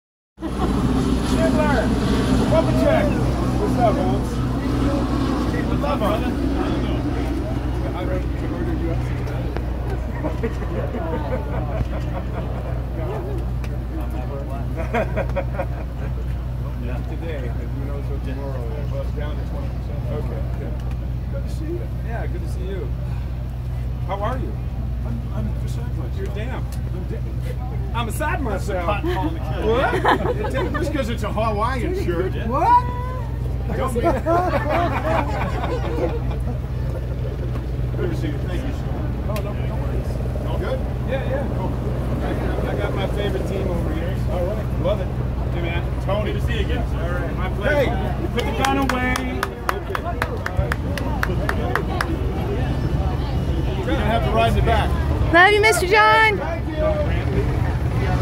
Duke Boys meet again! John Schneider Tom Wopat🧡
John Schneider Bo Duke greets Tom Wopat cousin Luke Duke at John Schneider Studios in Holden, LA for Bo's Extravaganza. Tom Wopat signed autographs, posed with fans, and sang in a concert. Dukes of Hazzard is real family.